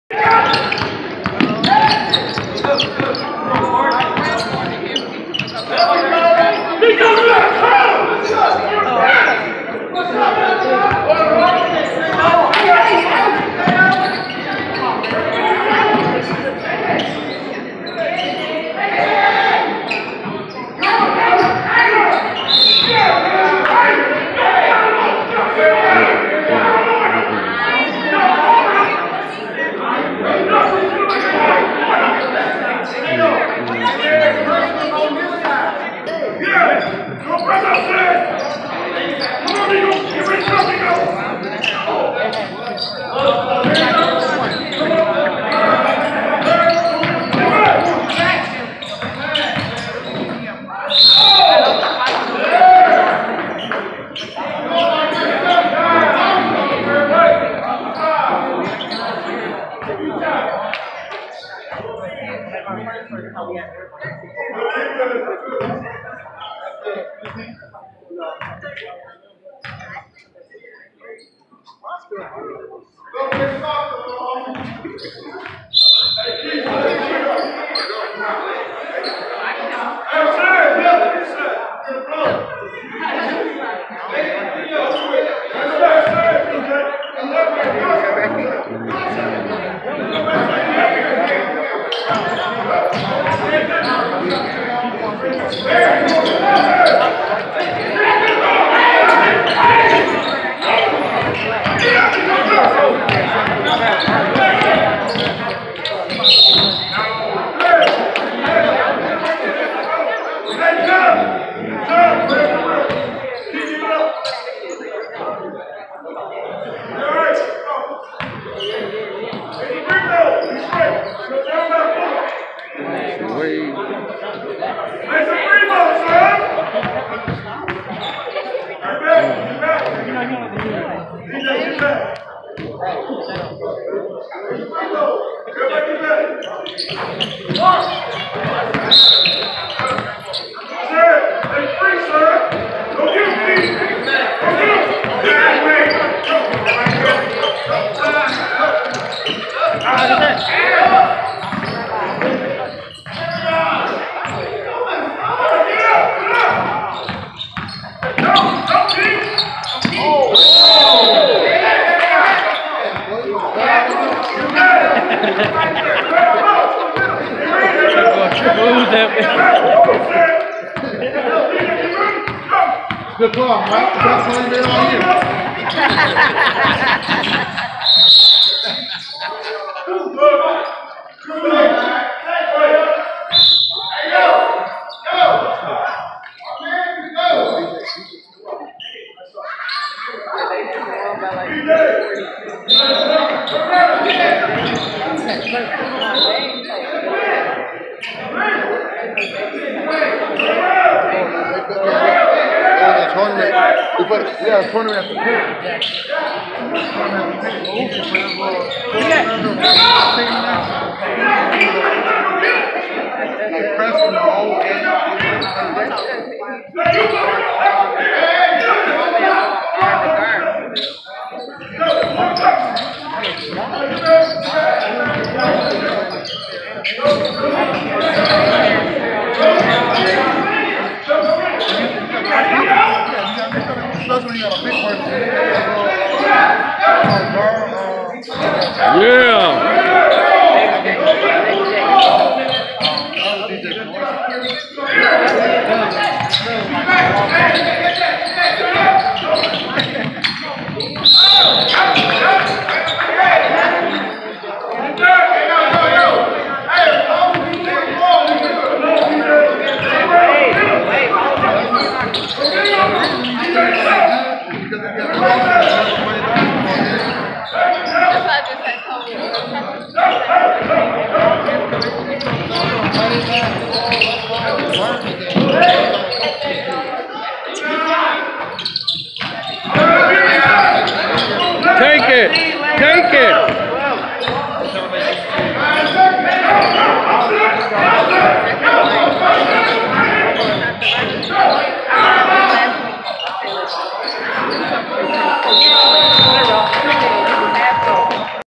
BMO Jr 16u Vs Stop The Violence 2nd Game Part 1
7th-10th grade BMO Jr squad looking tough...imagine if we had 5 practices. That would be one helluva sight.